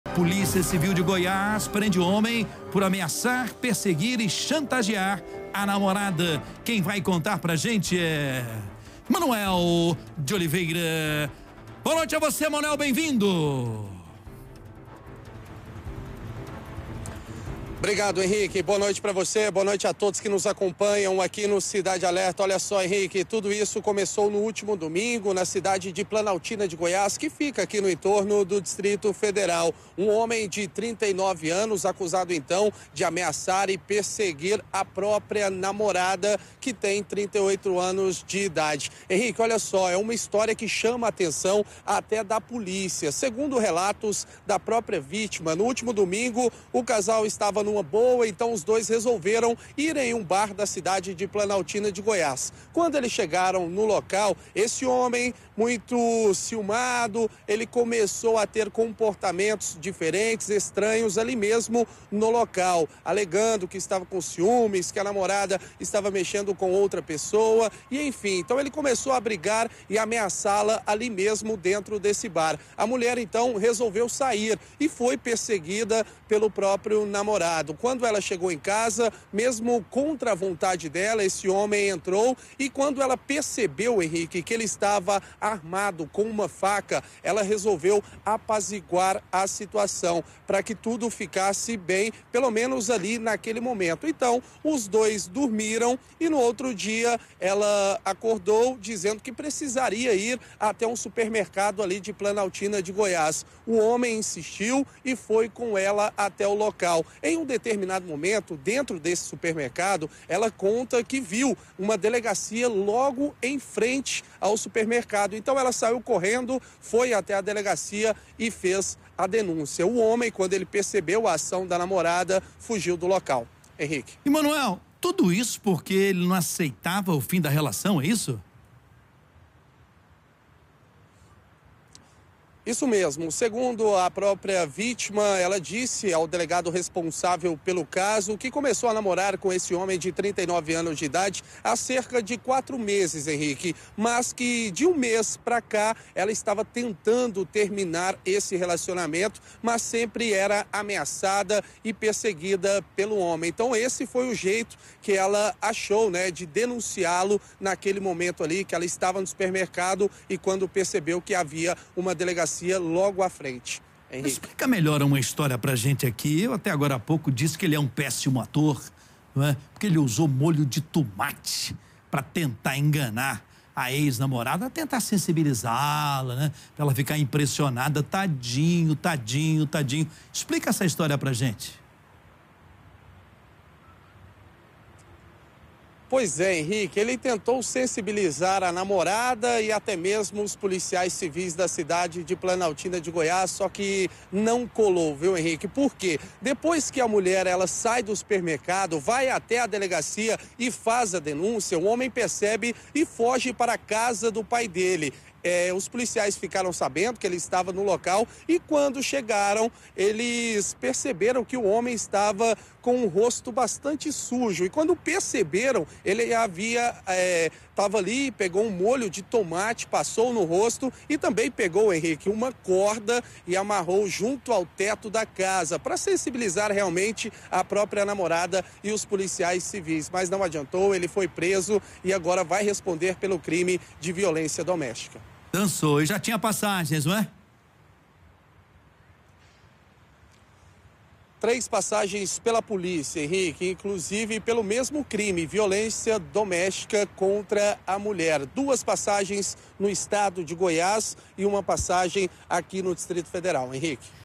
Polícia prende suspeito de ameaçar, perseguir e chantagear namorada
Um homem de 39 anos de idade foi preso suspeito de perseguir, ameaçar e chantagear a namorada, de 38 anos de idade, em Planaltina de Goiás, no Entorno do Distrito Federal. De acordo com a Polícia Civil, o homem foi encontrado com o rosto e parte do corpo com molho de tomate e uma corda pendurada ao lado, na tentativa de comover a vítima, após ela denunciá-lo. A mulher relatou que, ao chegar na própria residência, o homem permaneceu na casa contra a vontade dela.